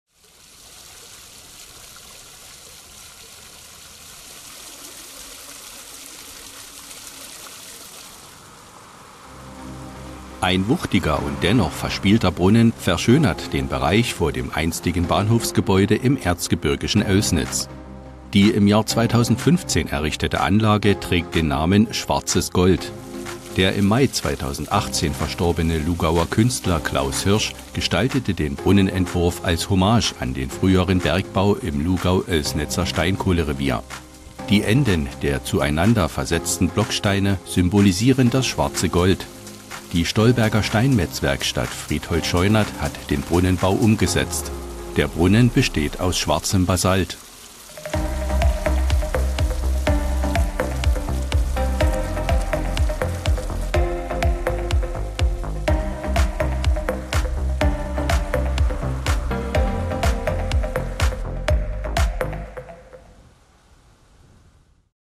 Wasserspiel (11)
OELSNITZ/ERZGEB.: Ein wuchtiger und dennoch verspielter Brunnen verschönert den Bereich vor dem einstigen Bahnhofsgebäude im erzgebirgischen Oelsnitz.